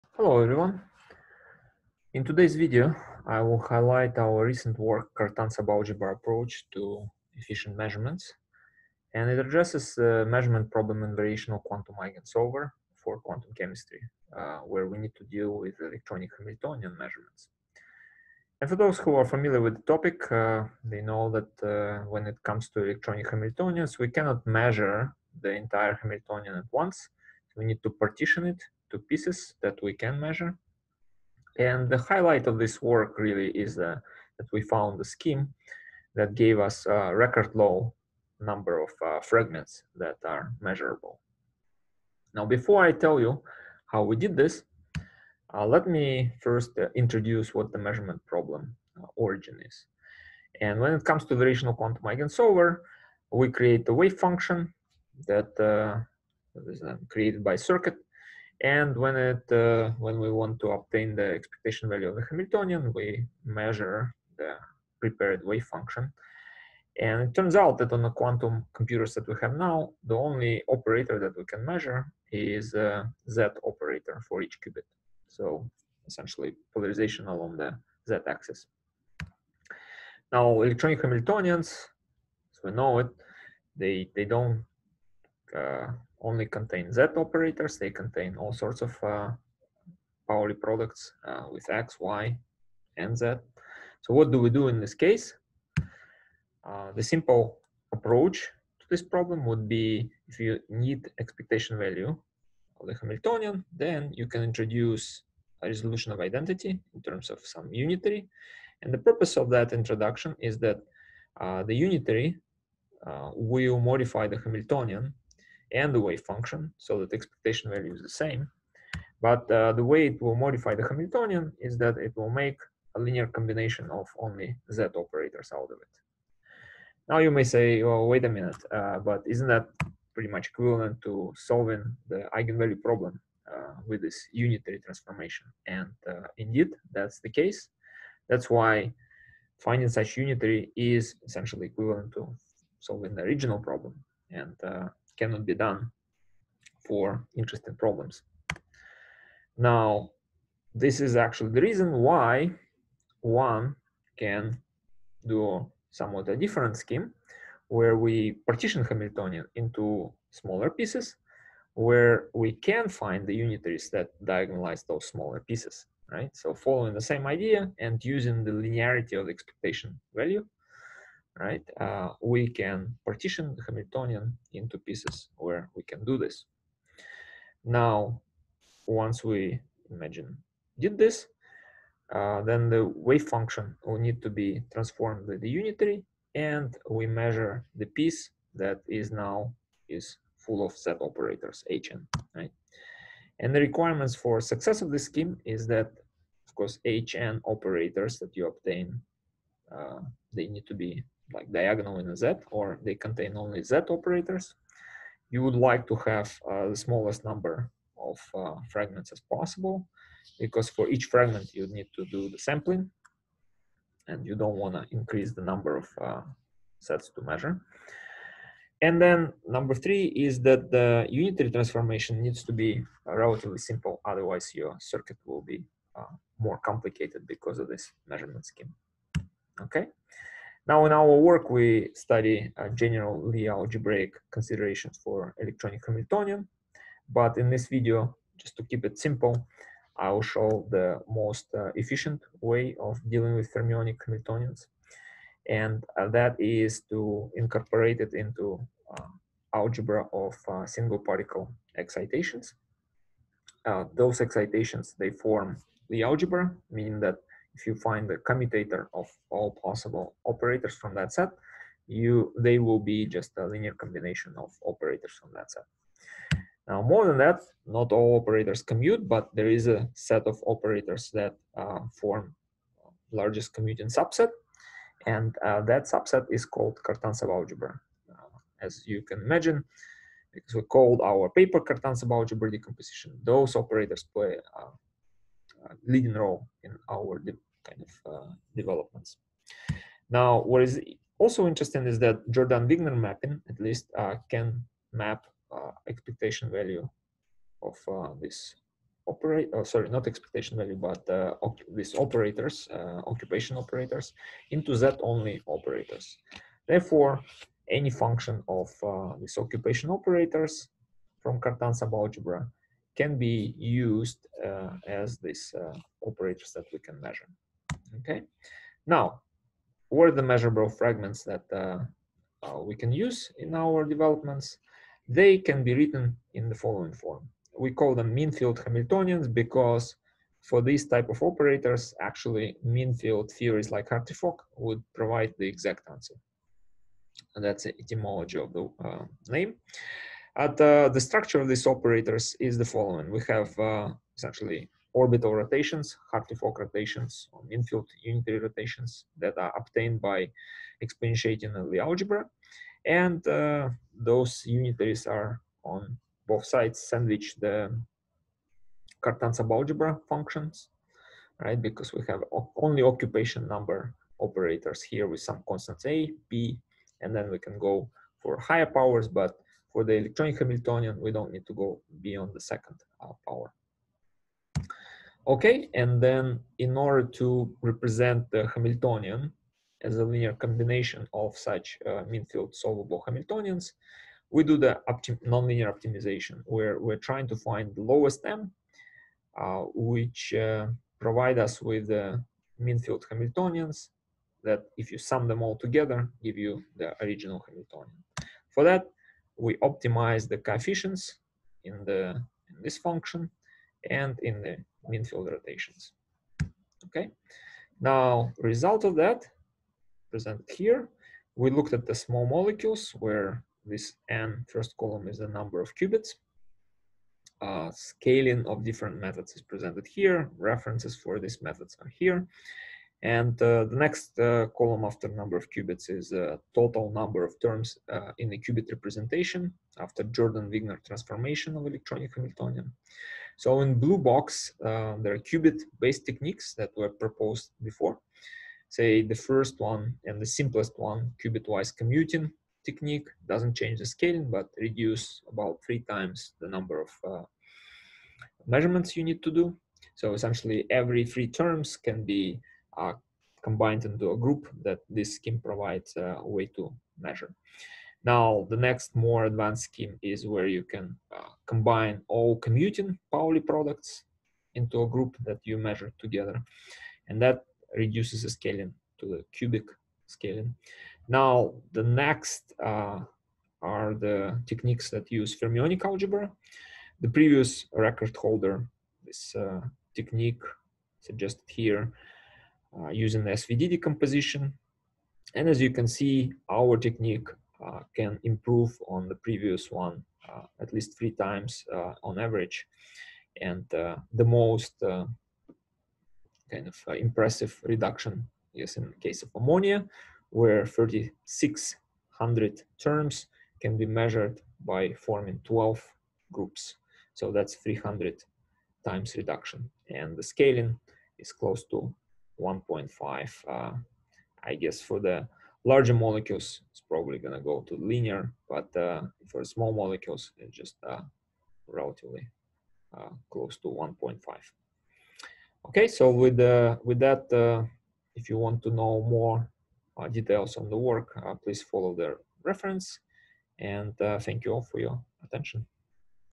Cartan sub-algebra approach to efficient measurements of quantum observables (arXiv:2007.01234)
Quantum Computing; Quantum Chemistry; Quantum Measurement; Variational Quantum Eigensolver